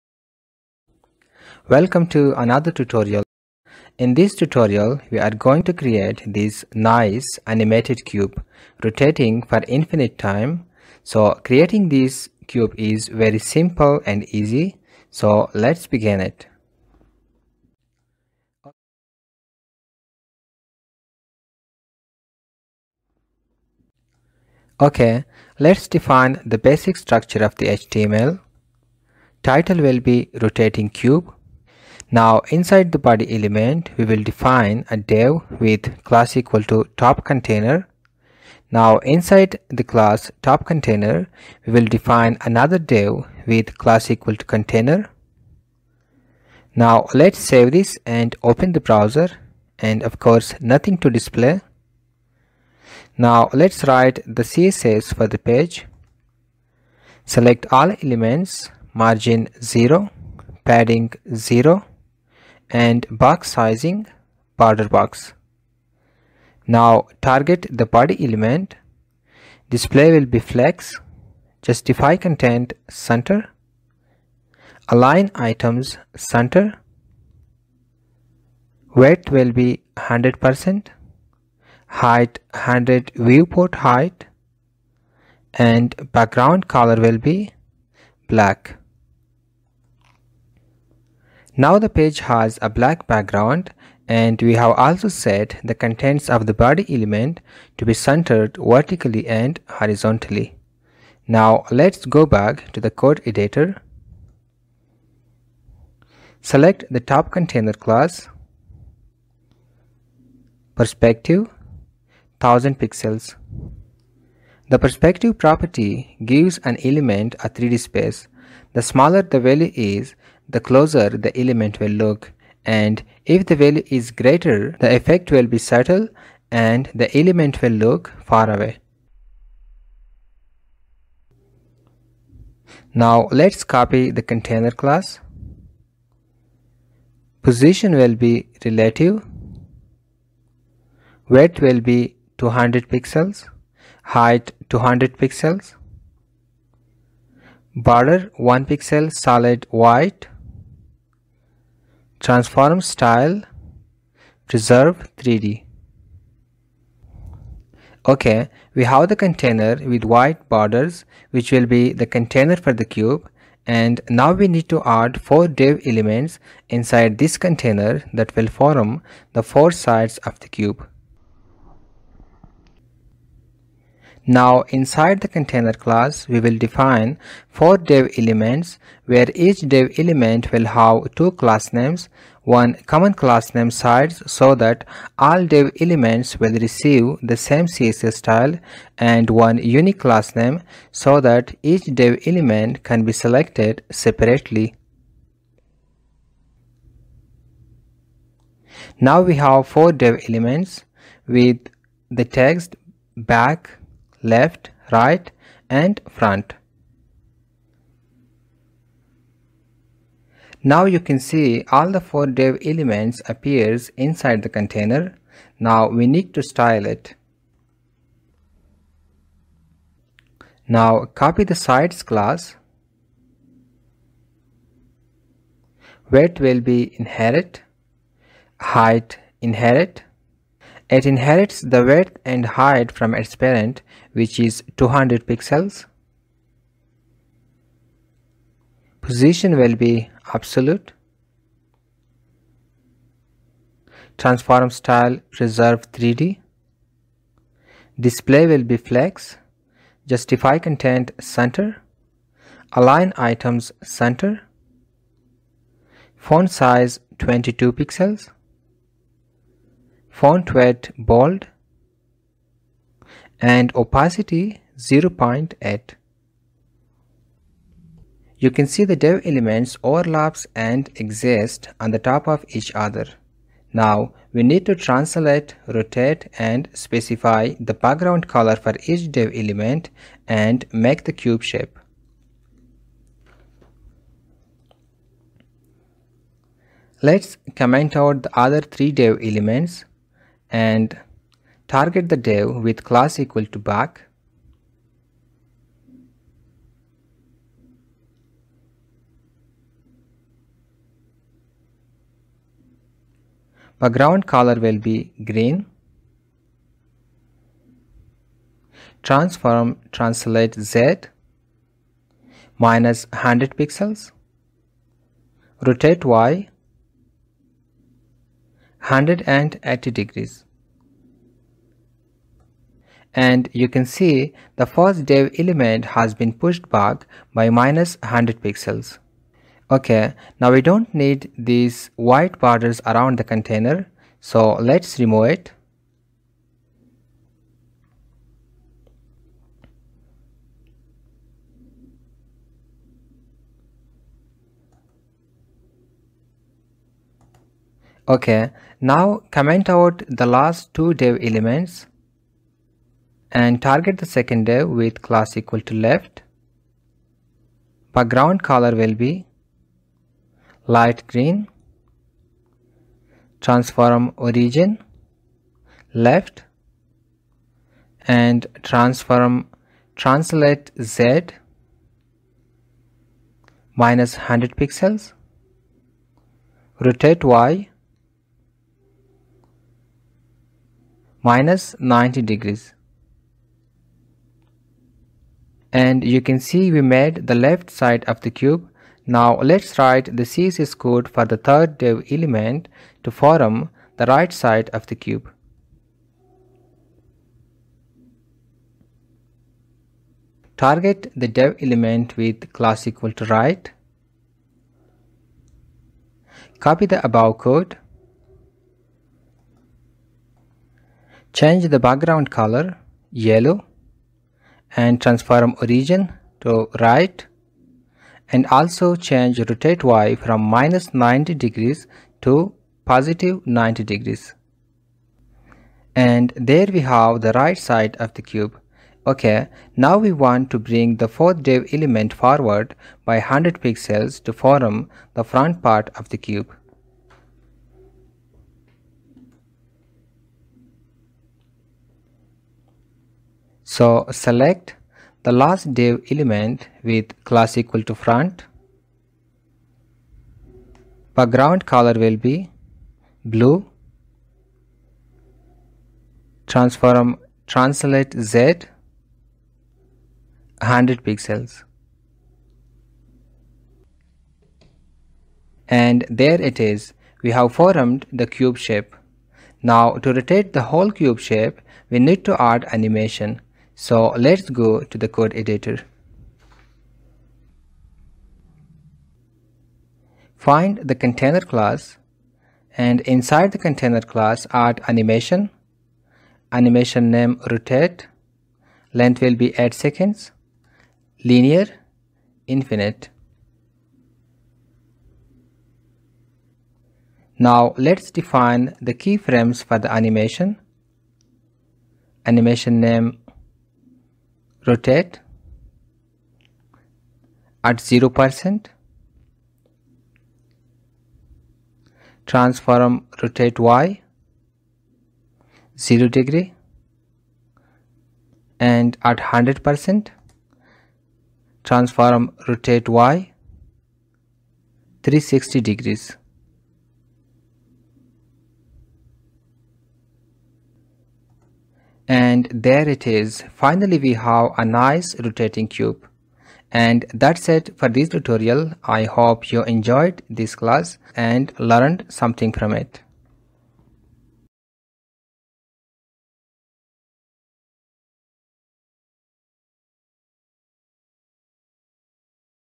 3D Rotating Cube Animation With HTML & CSS | CSS 3D Animation Tutorial
Hello friends!
In this video we are going to create 3d rotating cube animation using html and css.
we will use simple html and css code to make the 3d cube and you will also get familiar with some advance css properties like, perspective, transform-style, translate-Z, and rotate-y.
I hope this tutorial will be helpful for you and you will learn something new.

Ralated Keywords:

css 3d cube animation
css 3d cube rotating
rotating cube
css animation
css 3d animation